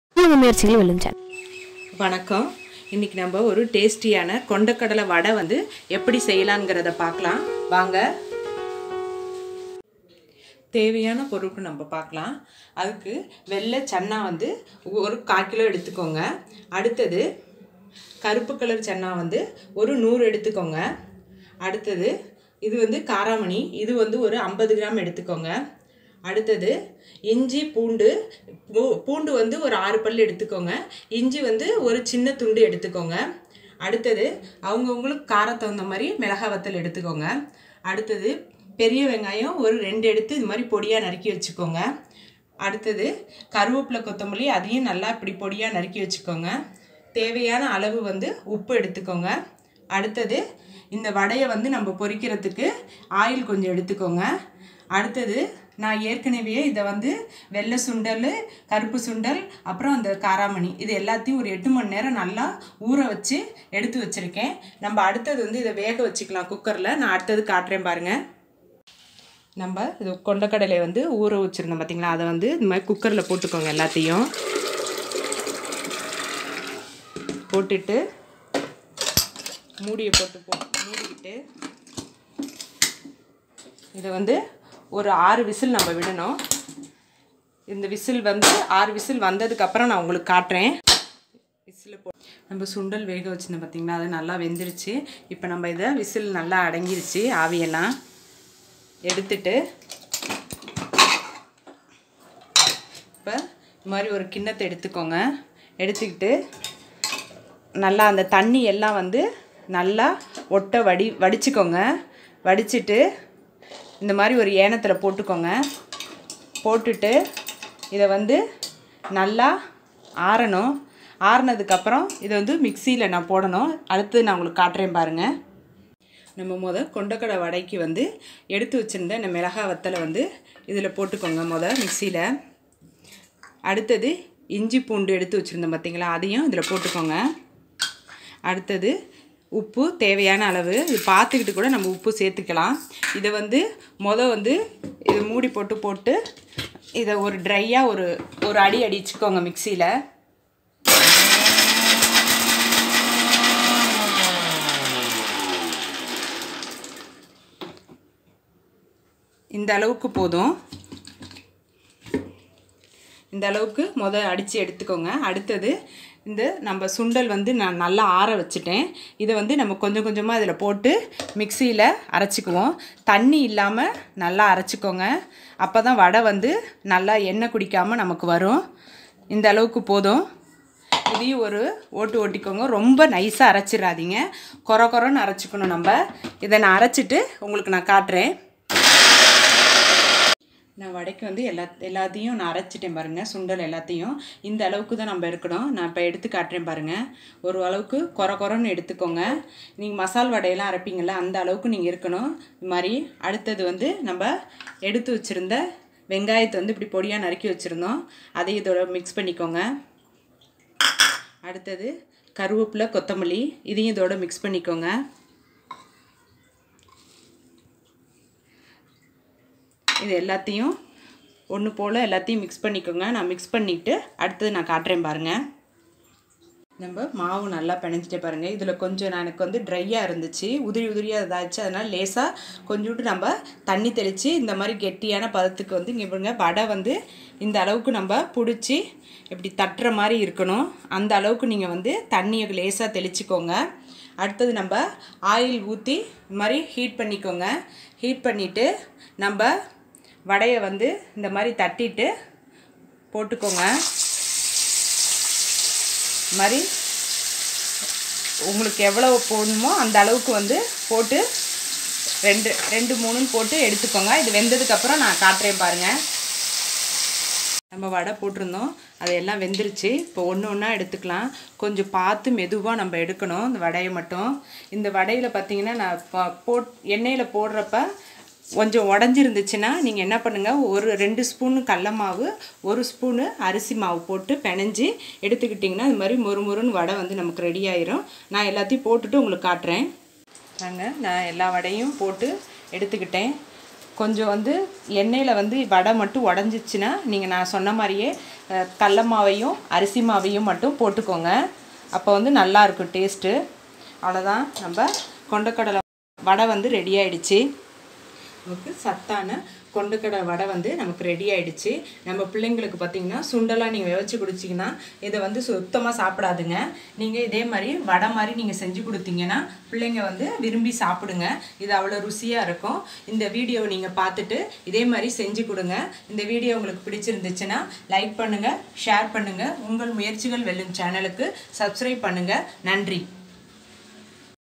Kondakadalai Vadai | கொண்டைக்கடலை வடை | How to make Kondakadalai Vadai in Tamil | Channa Vadai
சுவையான கொண்டைக்கடலை வடை செய்வது எப்படி என்று தெரிந்து கொள்ளலாம். Come let us see How to make Kondakadalai Vadai in Tamil.